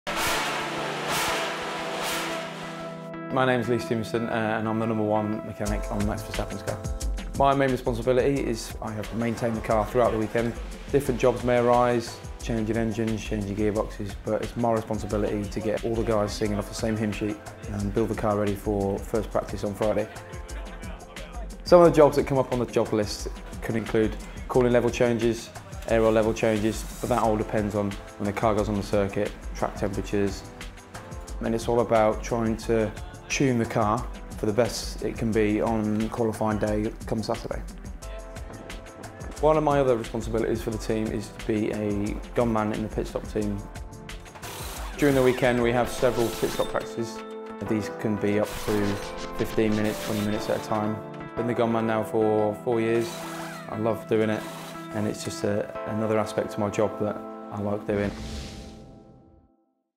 60 Seconds With: No.1 Mechanic Lee Stevenson
Max Verstappen's No.1 Mechanic, Lee Stevenson, takes us through a  typical day in the Red Bull Racing garage - in 60 seconds!